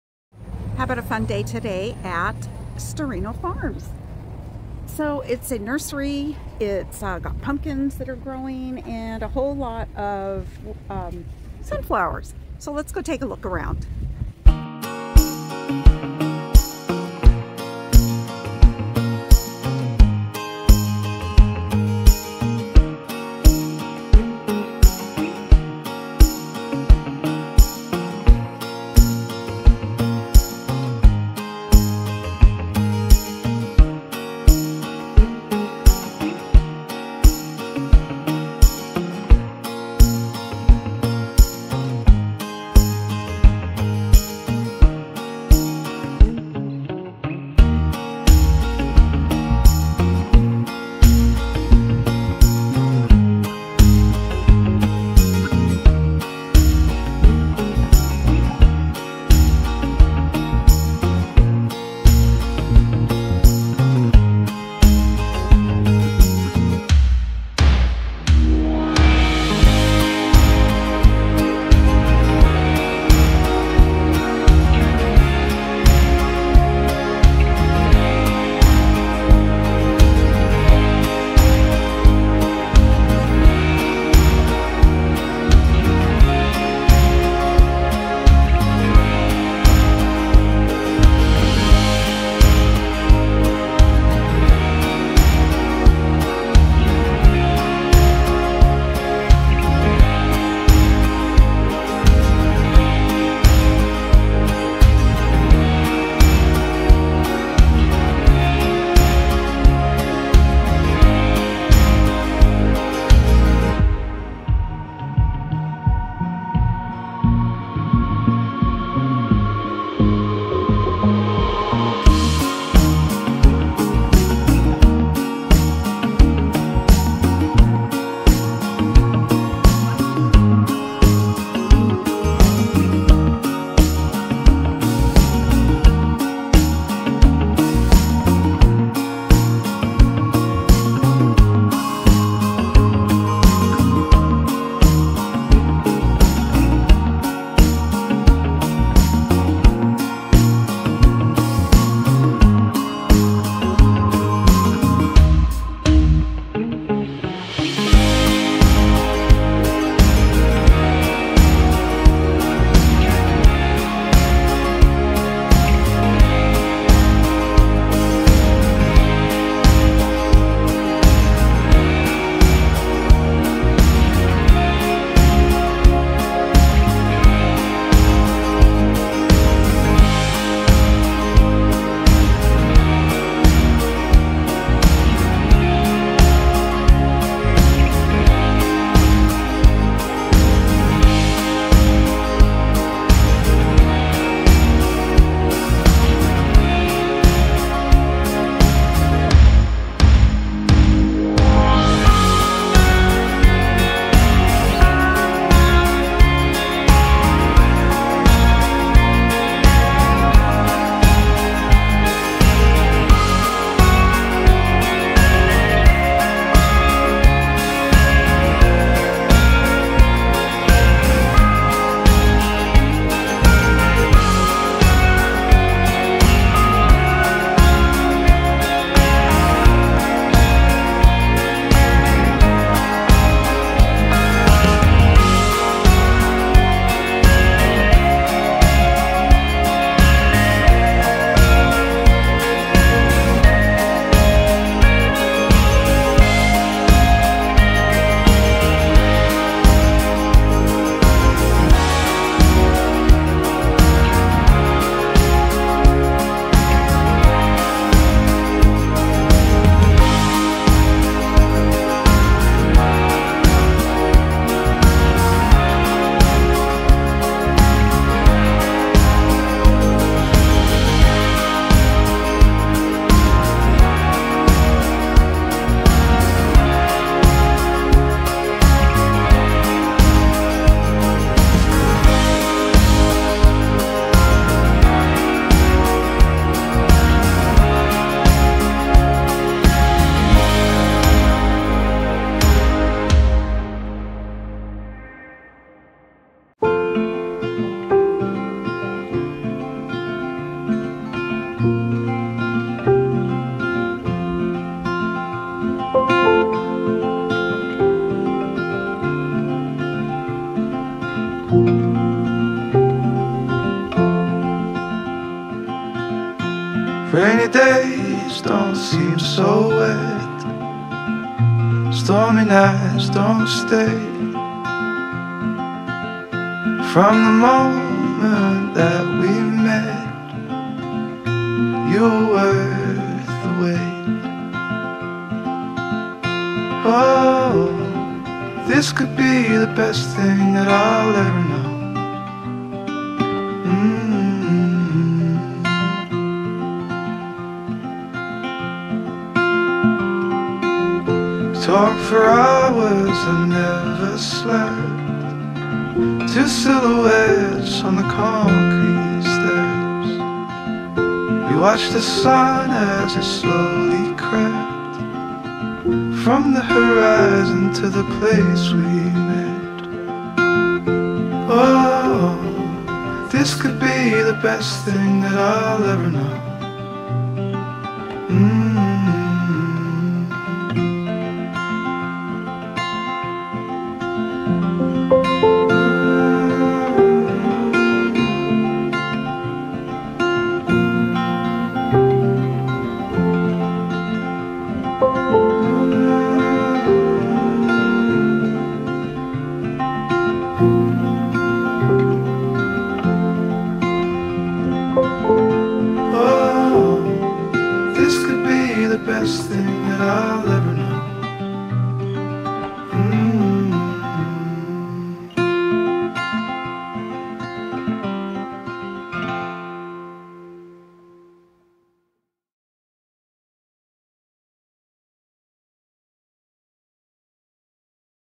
Vlog: Exploring a Farmer's Market | Pumpkins, Flowers and produce
Join me as I take you on a fun trip to a local farmer's market filled with the colors and flavors of fall! 🍂 From bright orange pumpkins, seasonal flowers and greenery, to a beautiful bounty of fresh fruits and vegetables, this market has it all. If you love a farmer's market vibe, this vlog is for you! 🌻🥕🍎